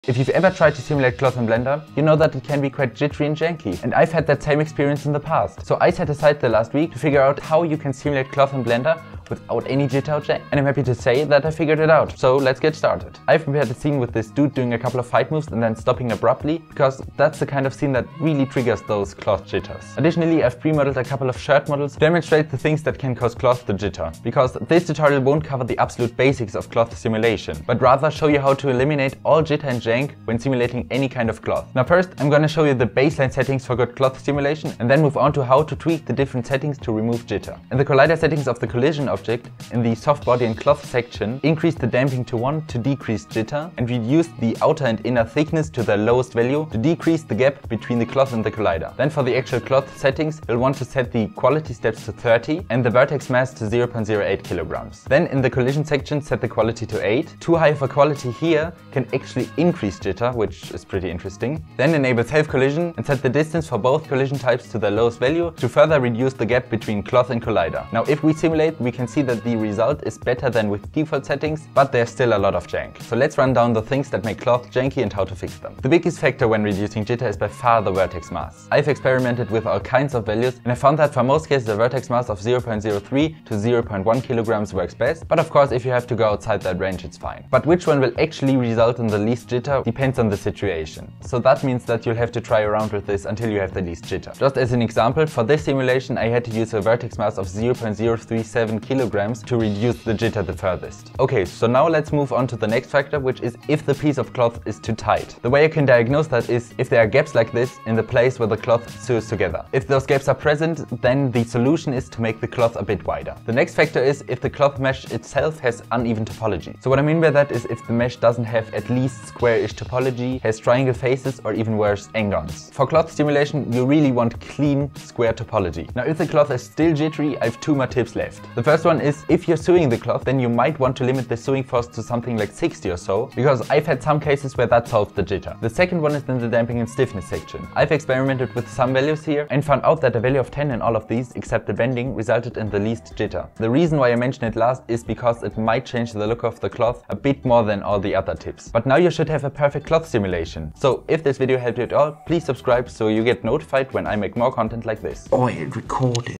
The SECRET of Perfect Cloth Simulations | Blender Cloth
In this video, I'll show you how to create a perfect cloth simulation in blender, and how to solve jittering cloth simulations in blender.

+ on request, I can also send you a testimonial